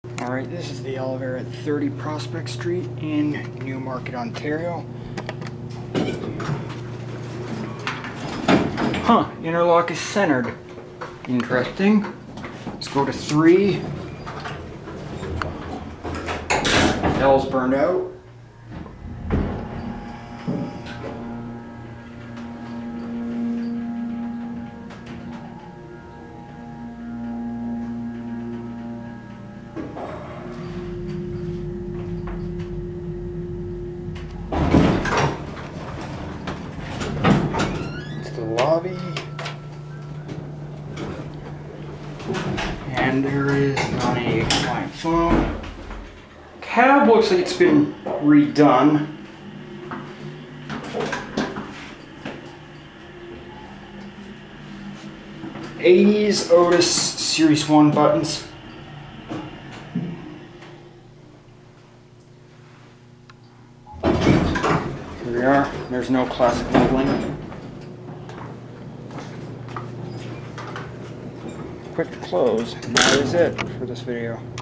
Otis Series 1 Hydraulic Elevator @ 30 Prospect Street, Newmarket ON
Of all of the older Otis Series 1 elevators that I have filmed in Newmarket, this one is probably in the best shape out of all of them. I thought that the cab may have been redone because the walls don't look grungy in the slightest, and usually fake wood Otis cabs from the 1980s do not age very well.